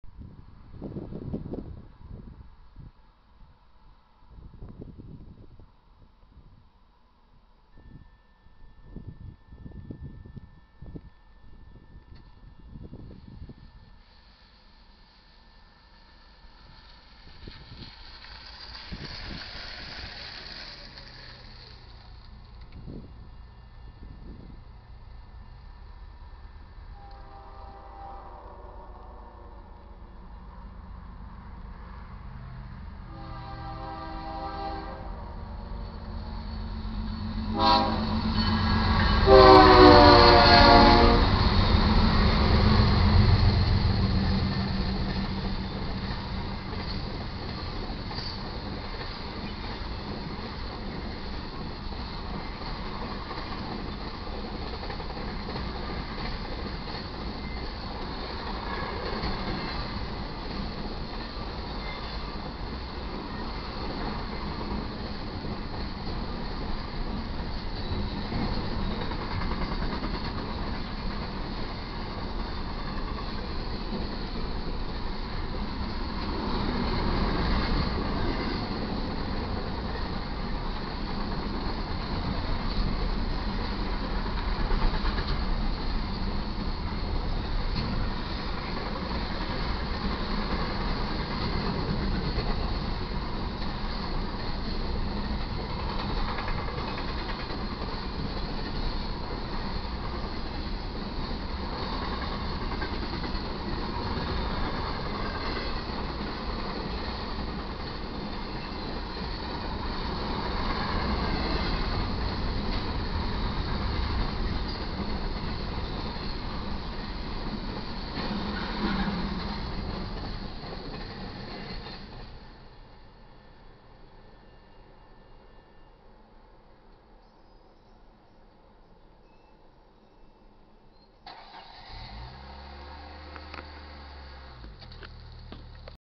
BNSF 7668 West Z Train (11-22-2013)
West Z Train/Main Track 1/M. P. 393.27
BNSF St. Croix Subdivision/11:07 am